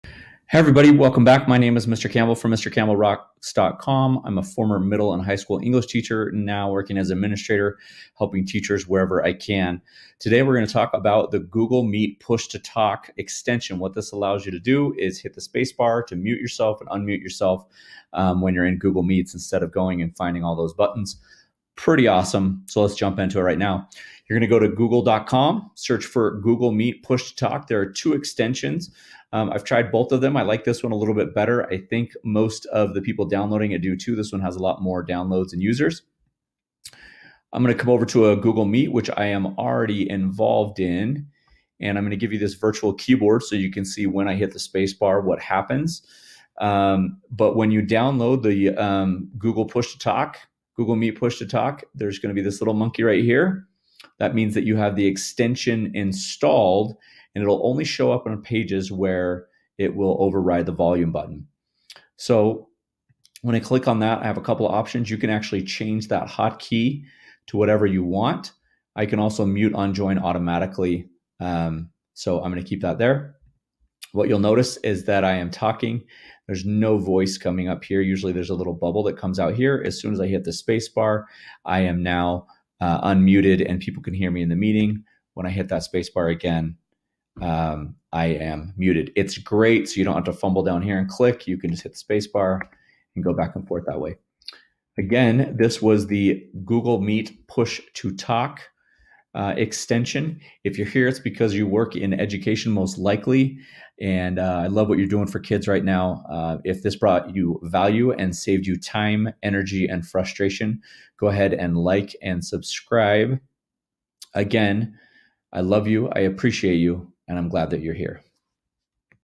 Google Meet: Push to Talk Extension
Find other time saving extension tutorials here.

Whiteboard Google Meet: Push to Talk Extension allows those that use the extension to create hot keys, which allows participants to mute and unmute themselves with a click of a button. For most users this will be the space bar.